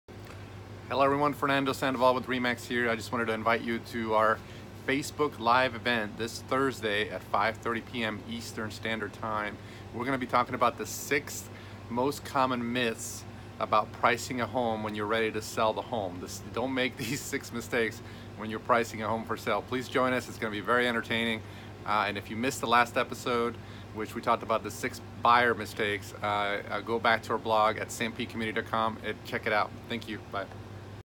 Avoid These 6 Seller Myths When Pricing a House for Sale
Invitation to Live Broadcast on Facebook 11/8/18

after this date you can check out the recorded broadcast on our blog on our web site by following this link:

rem.ax/ourblog